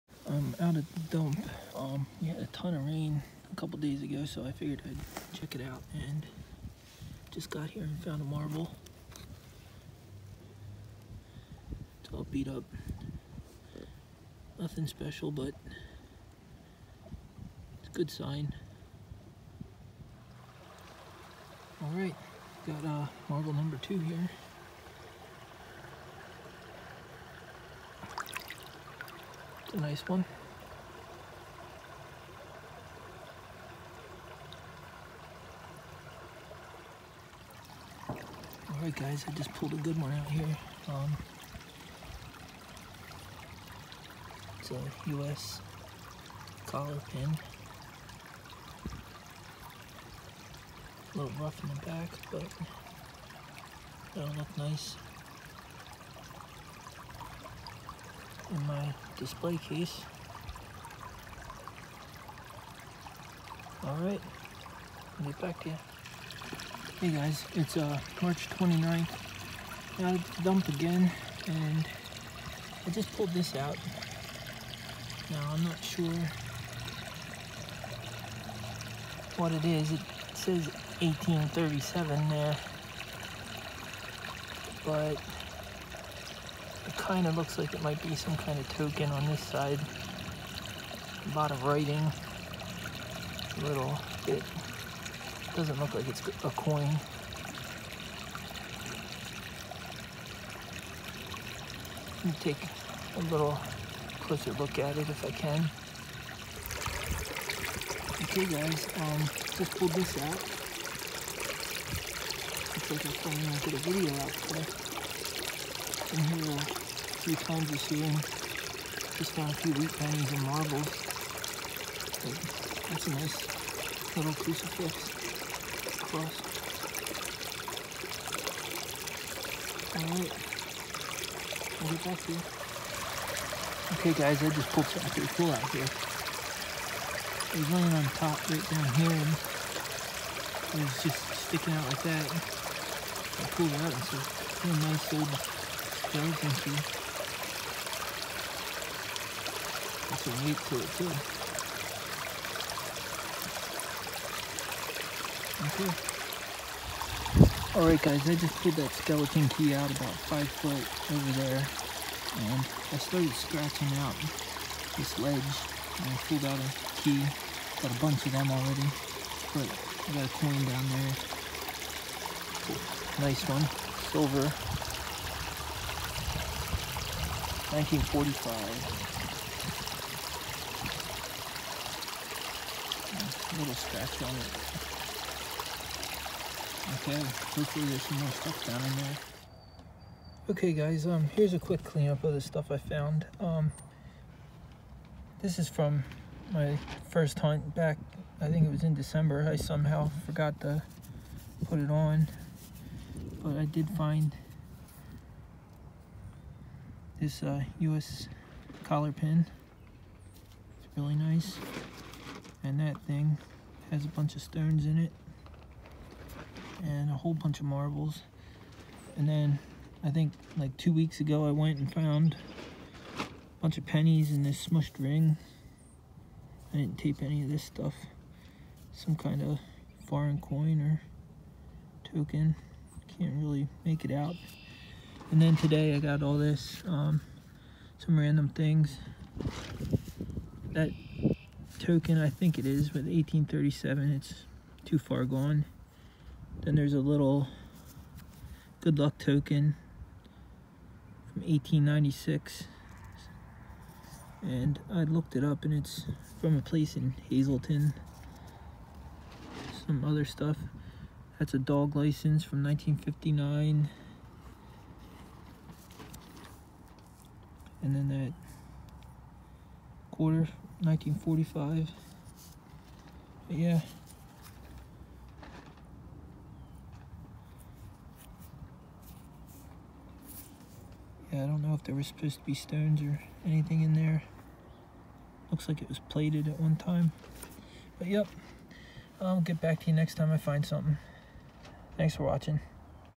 Back At The Dump
A few recent hunts at the dump and I’m still finding some good things.